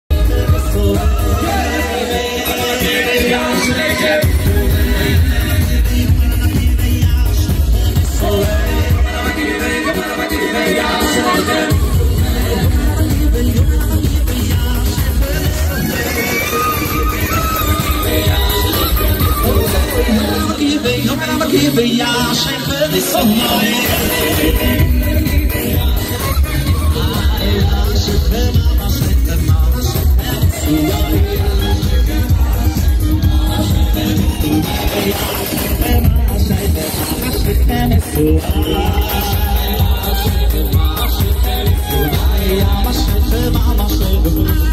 הריקודים בציון רבי נחמן מברסלב באומן ל"ג בעומר תשפ"א
תיעוד מהריקודים בציון רבי נחמן מברסלב באומן ל"ג בעומר תשפ"א.